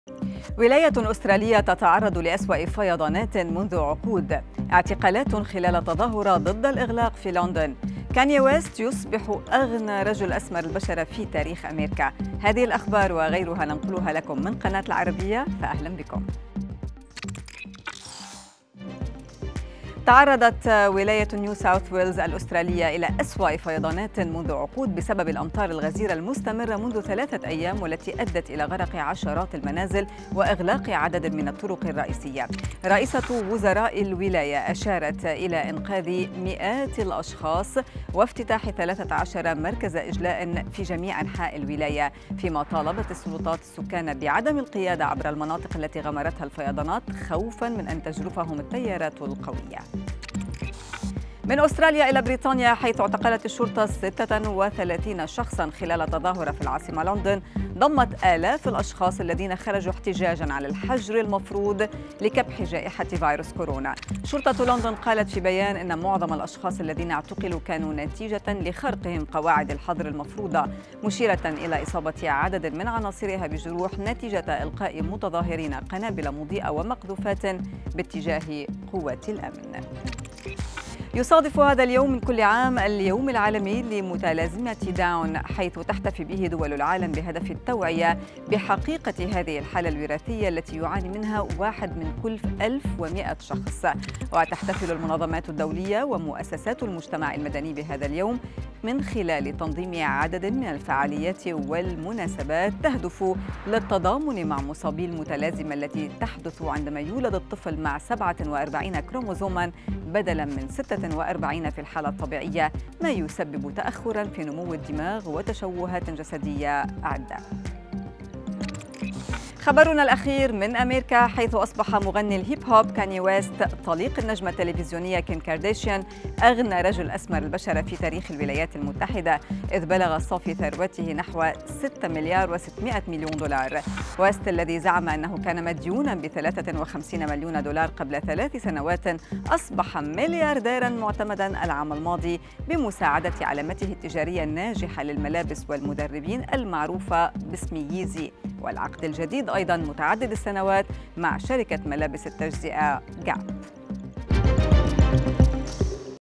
الأخبار في دقيقتين
☔️ ولاية أسترالية تتعرض لأسوء فيضانات منذ عقود
‏🇬🇧 اعتقالات خلال تظاهرة ضد الإغلاق في لندن
💵 كاني ويست يصبح أغنى رجل أسمر البشرة في تاريخ أميركا

ـــــــــــــــــــــــــــــــــــــــــــــــــــــــــــــــــــــــــــــــــــــــــــــــــــــــــــــــــــــــــــــــــــــ
تيلغرام
سناب شات alarabiya
تفاعلكم
صباح العربية
بانوراما
العربية الليلة
Live Alarabiya
Alarabiya Live